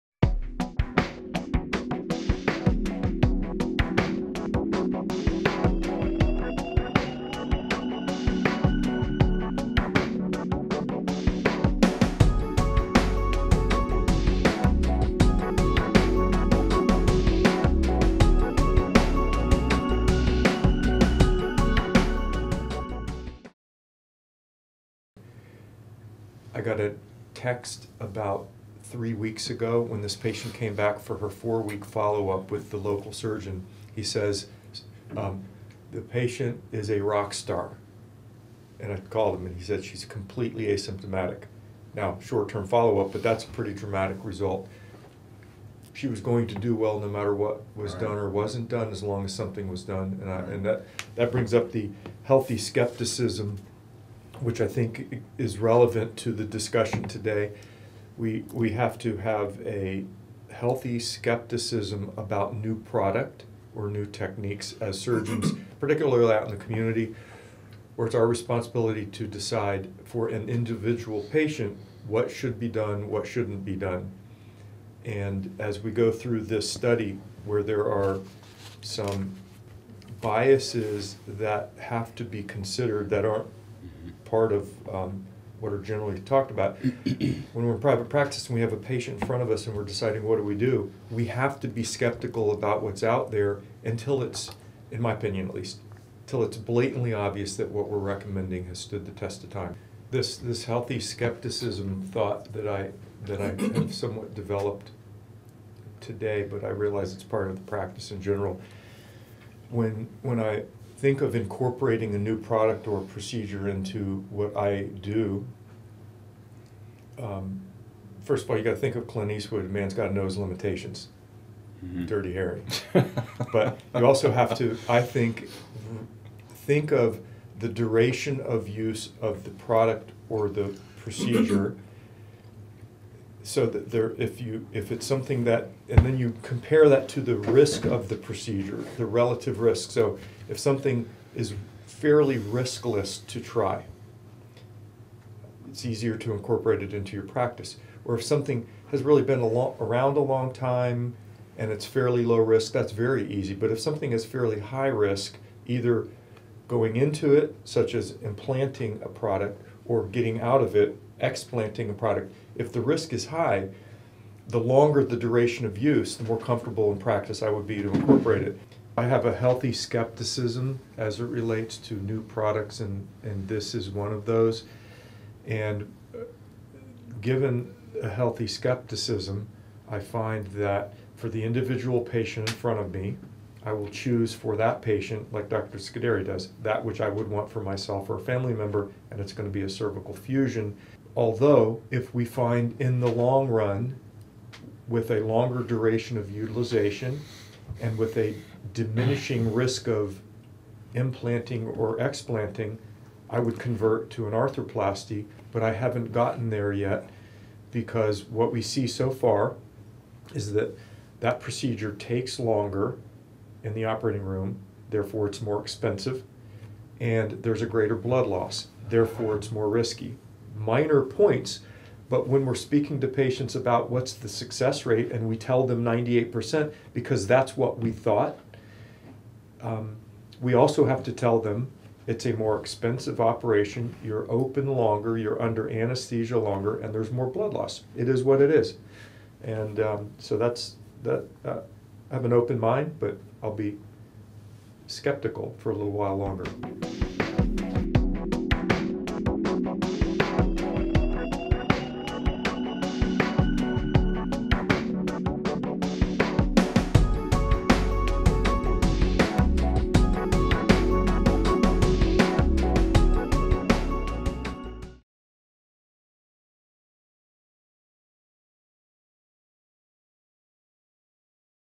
Healthy Skepticism - Cervical Arthroplasty vs. Cervical Fusion
Commentary by David Campbell MD
Excerpt taken from the Spine Minds topic: "Devices, Trends and Materials"